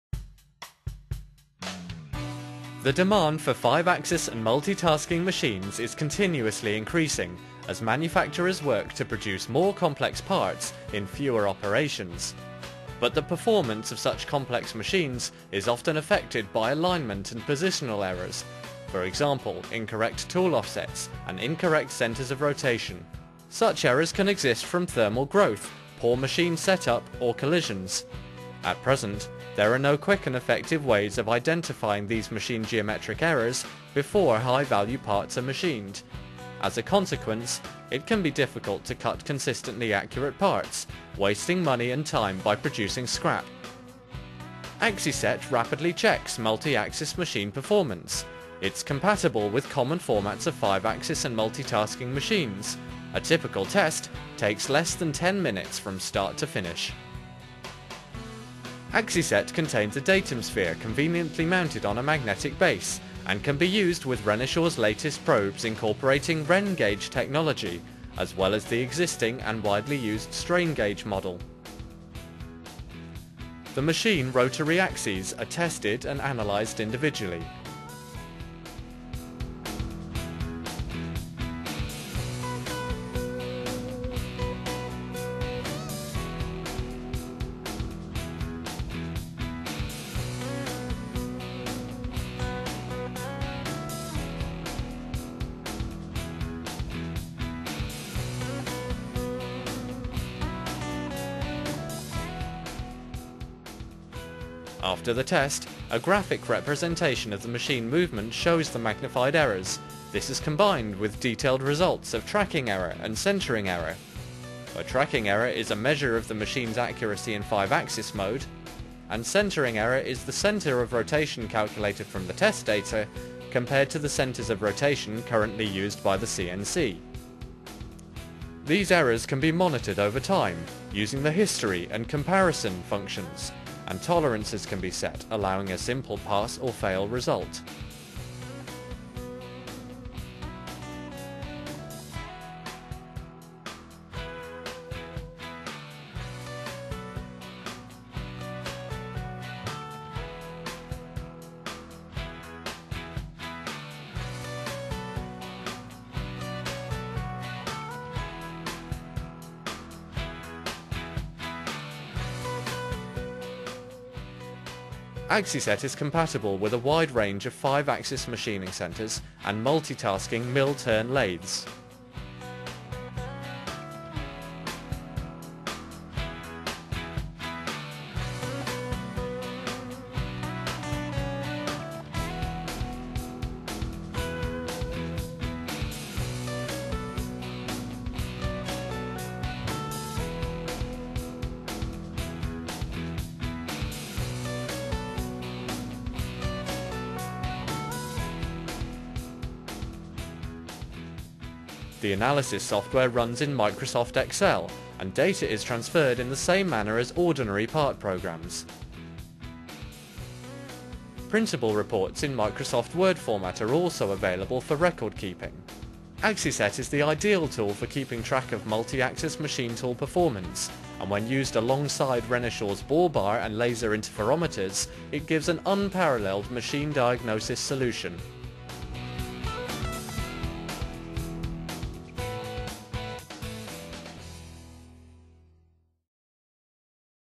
AxiSet™ Check-Up Movie
Compatible with common formats of 5-axis and multi-tasking machines, AxiSet™ Check-Up provides machine users with a fast and accurate health check of rotary axis pivot points. Alignment and positioning performance checks are carried out rapidly to benchmark and monitor complex machines over time.